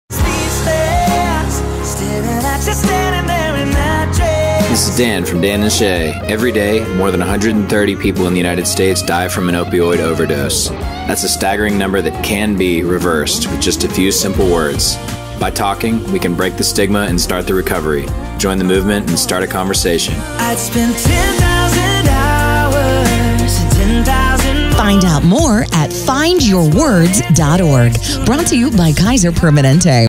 Help Dan and Shay fight stigma, raise awareness, and spread hope | Kaiser Permanente
Talking about mental health can be hard. It’s time to make it easier — and empower more people to ask for help, take action to help themselves, and support others. Visit FindYourWords.org for tools and resources to start the conversation.